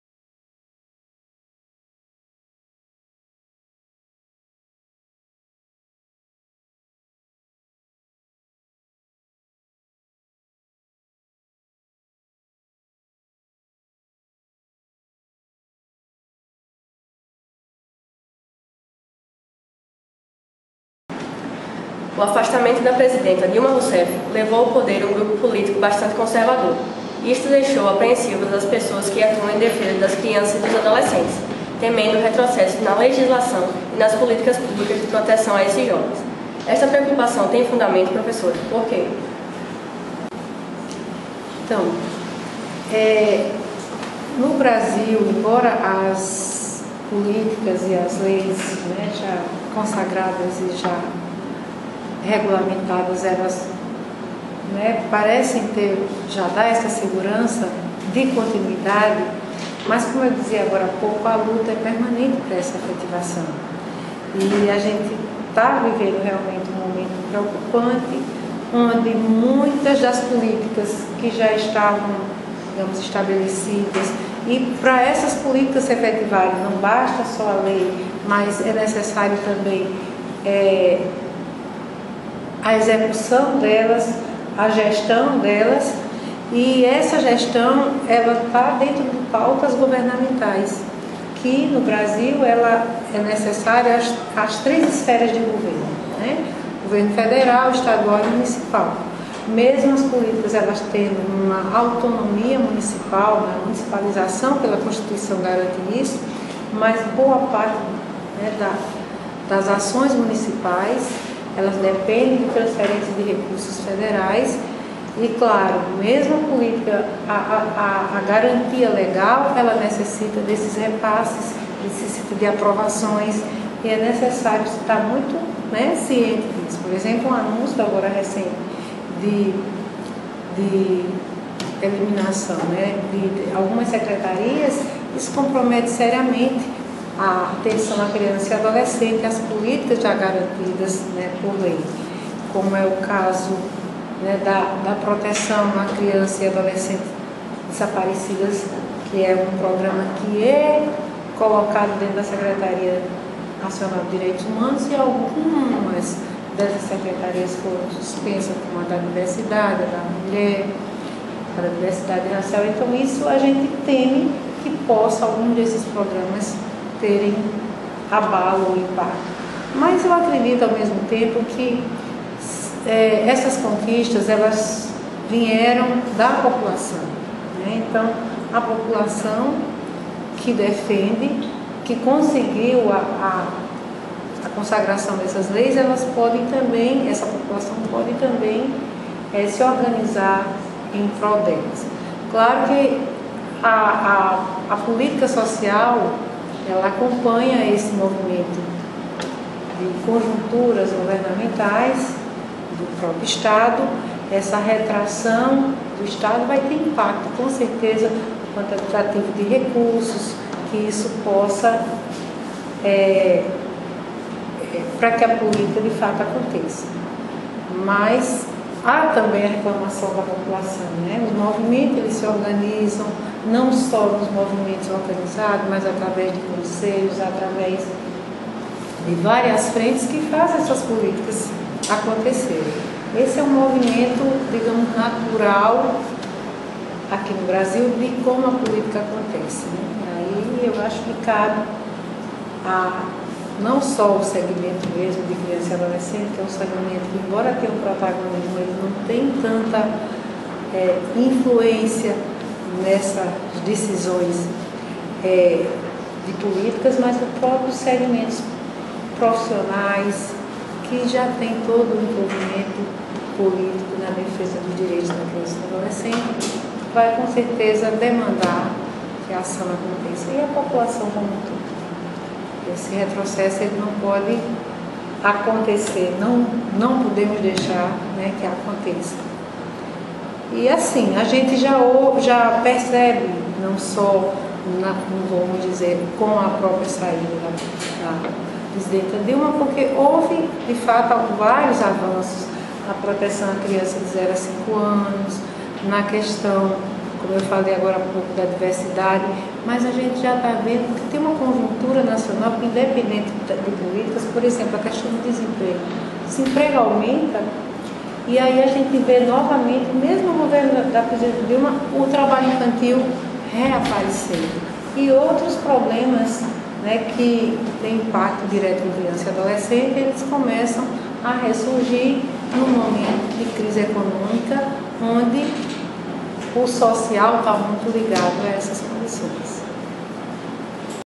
IV Colóquio sobre Infância e Adolescência, com Maria do Socorro de Souza Vieira – 2
2 - Política e a proteção à infância e à adolescência
A convidada fala sobre a relação entre as mudanças políticas e os possíveis impactos na legislação de proteção a crianças e adolescentes.
Segundo vídeo do Colóquio sobre Infância e Adolescência realizado no dia 25 de maio com a professora Maria do Socorro de Souza Vieira (Programa de Pós-Graduação em Serviço Social da UFPB) e organizado pelas alunas Heloysa Andrade, Jeane Pontes e Júlia Brito para a disciplina de Jornalismo e Cidadania do curso de Jornalismo da UFPB. Supervisão: Carmélio Reynaldo Ferreira.